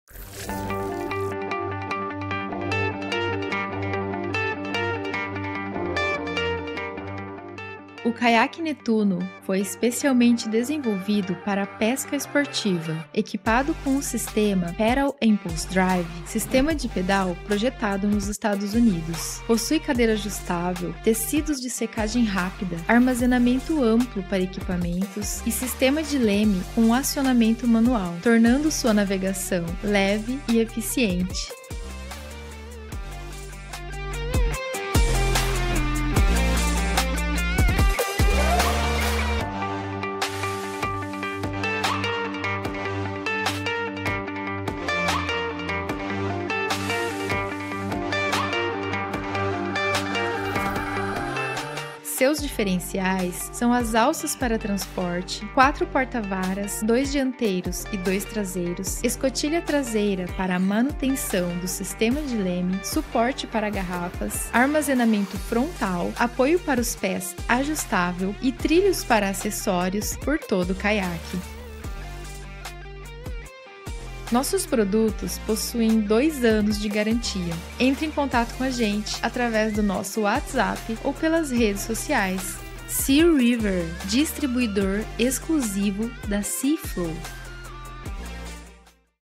Descubra o Caiaque Netuno: A Nova Era da Aventura Aquática
Prepare-se para uma emocionante jornada pelas águas com o lançamento da nossa linha de caiaques. Conheça as incríveis características e inovações que fazem o Netuno ser cada vez mais extraordinário, capaz de levar suas aventuras aquáticas a um patamar totalmente novo. Não perca a oportunidade de descobrir mais sobre essa adição emocionante à nossa família de caiaques. 🚣💦

 Caiaque Netuno:
Conforto de Classe ✅
Estabilidade Incomparável ✅
Durabilidade Excepcional ✅
Adequado para Todos os Níveis de Habilidade ✅
Design Moderno e Elegante ✅